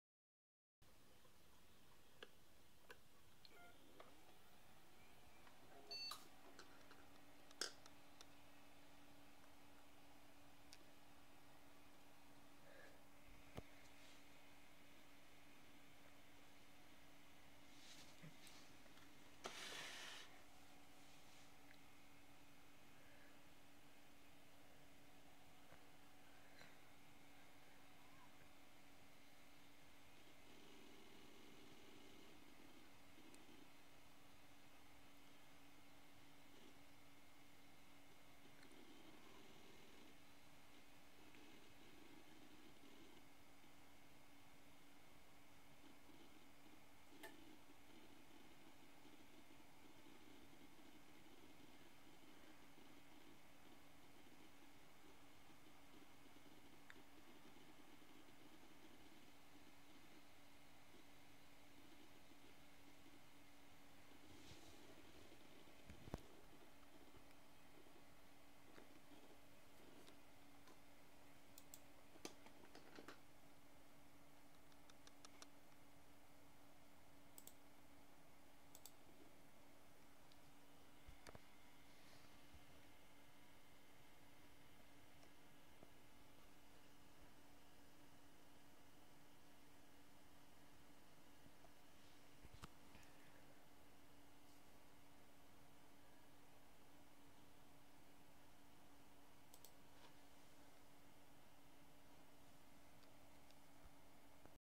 Windows 2000 booting on pentium 1 computer
Pentium 1 computer specification components AMD duron 133 mhz ram memory 128MB sdram hard disk maxtor 80 GB AGP cart 64 MB motherpad pentium